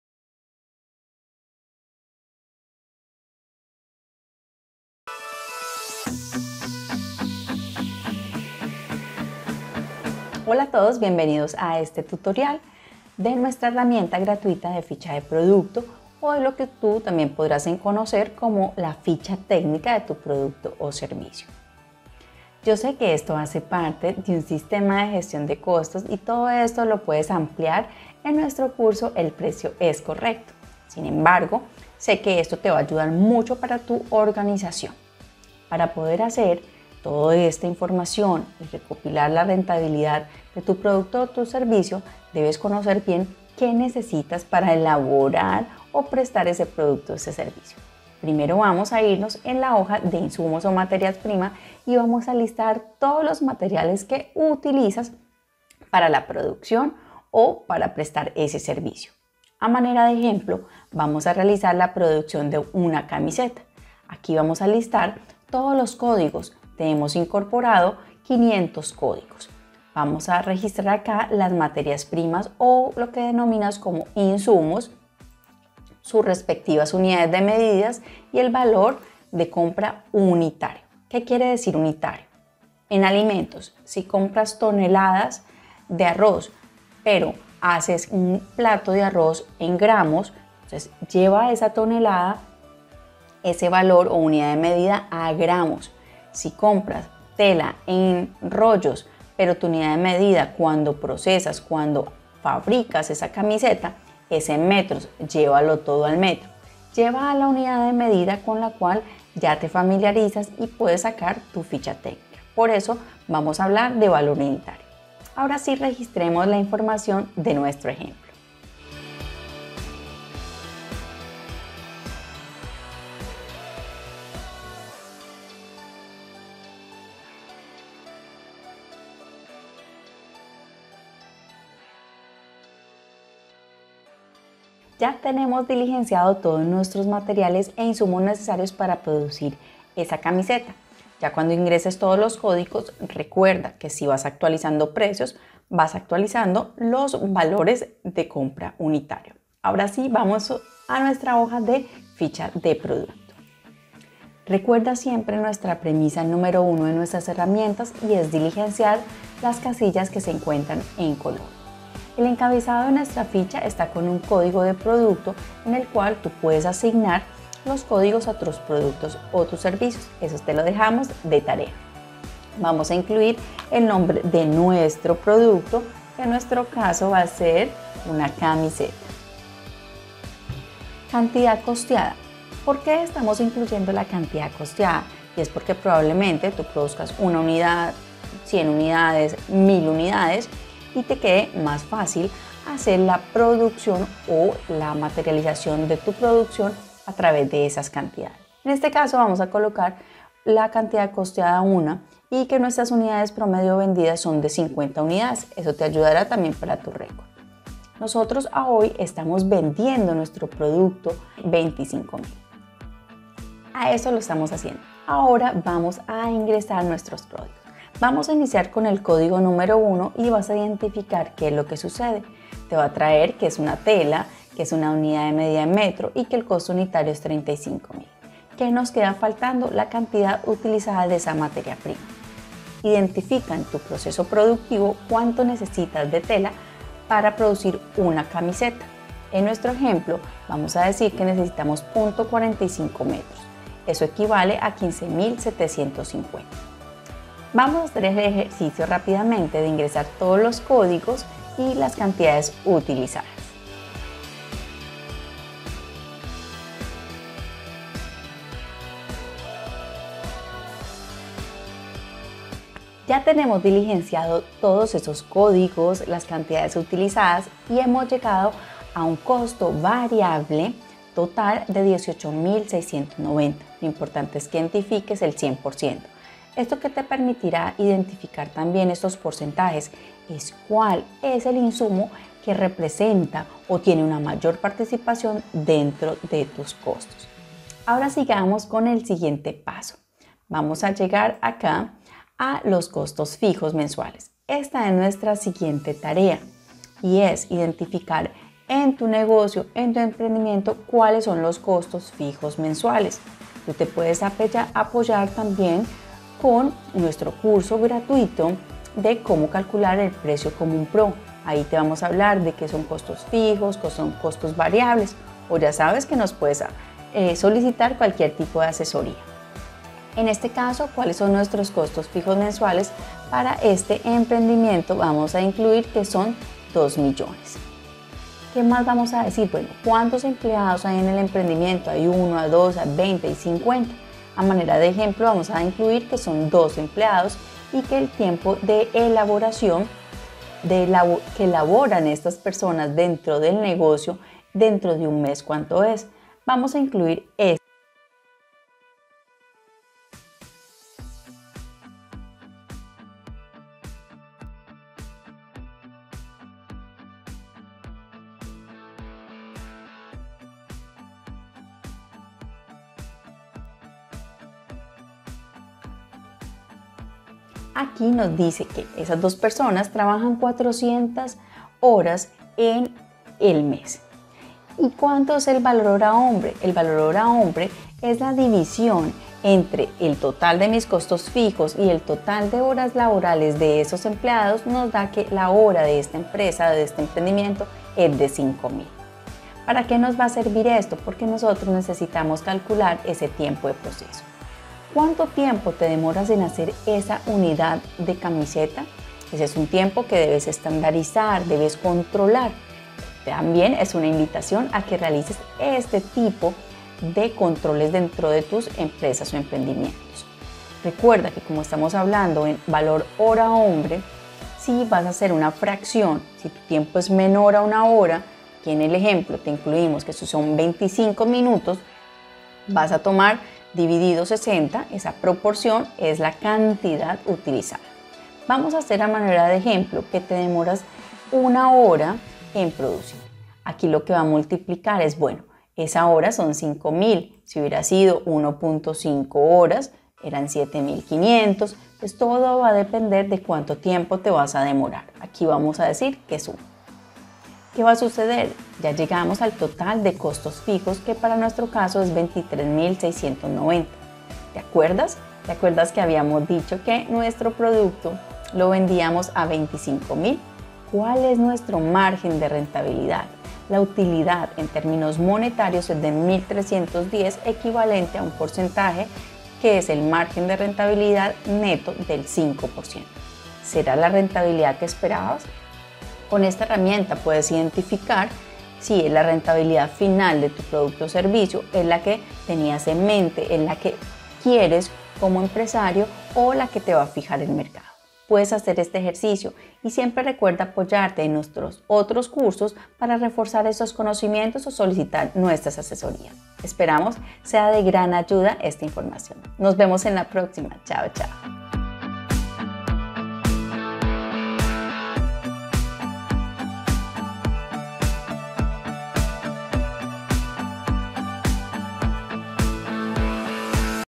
[Herramienta] - Ficha Técnica y de Rentabilidad de productos / servicios
Con esta herramienta lograrás organizar tu negocio para así obtener la rentabilidad de tus productos y/o servicios que tanto has deseado..

🔵 Calcula el precio correcto de tus productos o servicios con la Guia y Herramientas de nuestro Curso